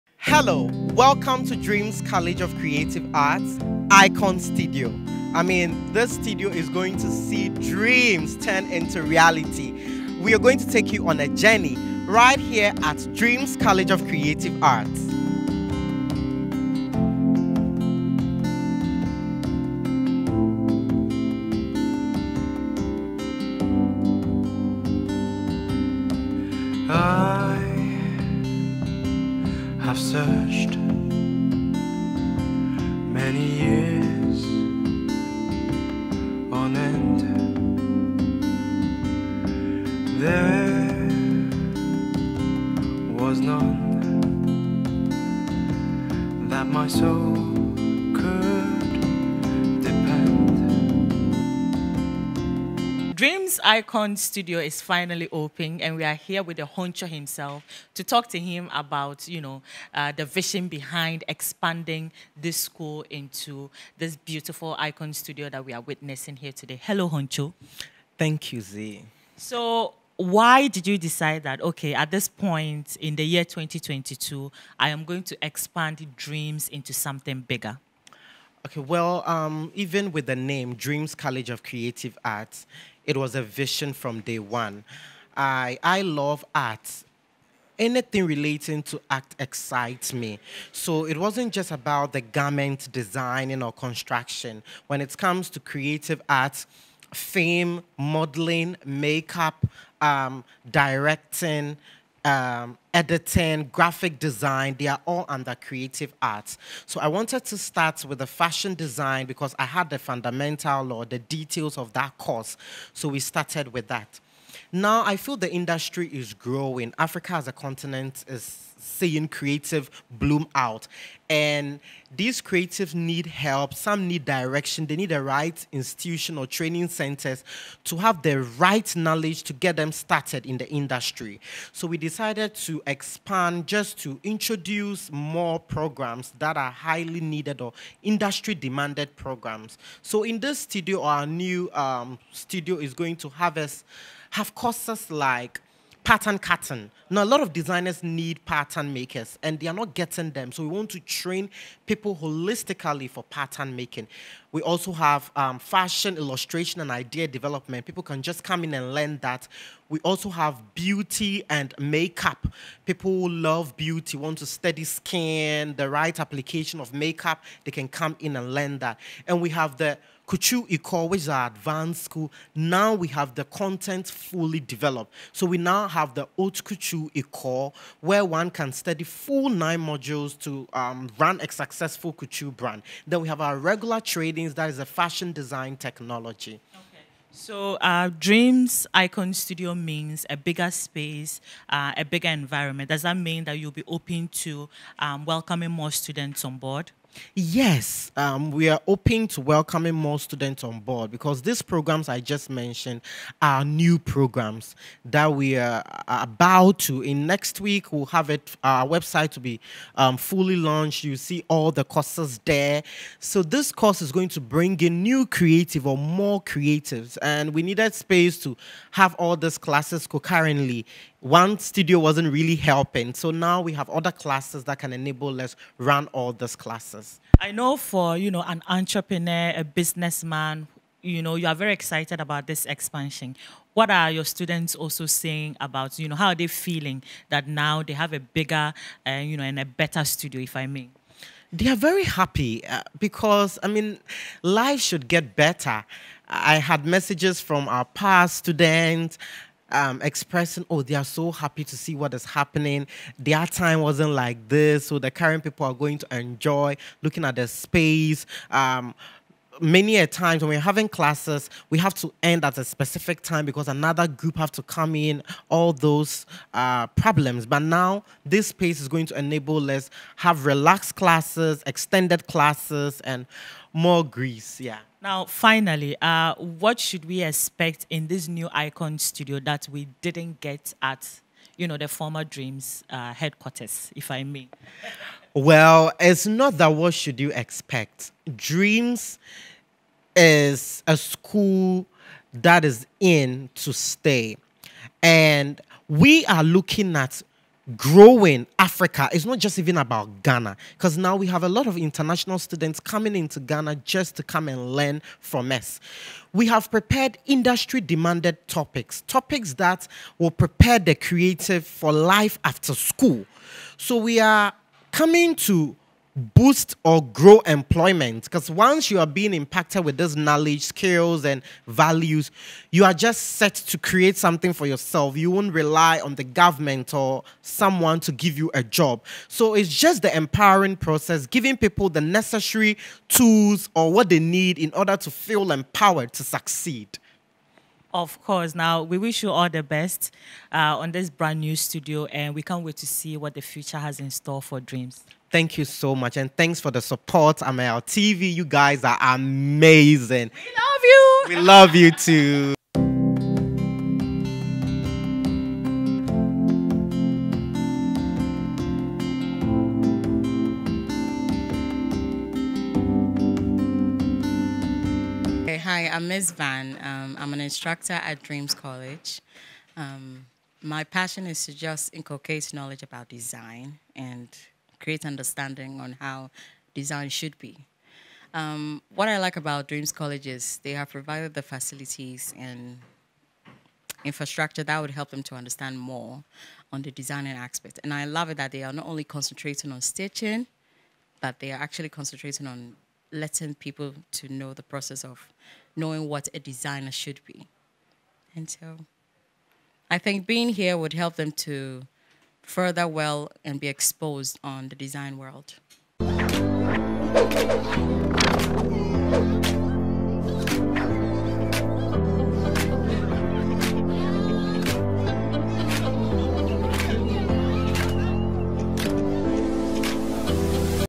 Dream College of Creative Arts outdoors a luxurious studio space dubbed " Icon Studio"
Ameyaw TV brings you up to date with news, exclusive interviews, live shows, events, and lifestyle shows among others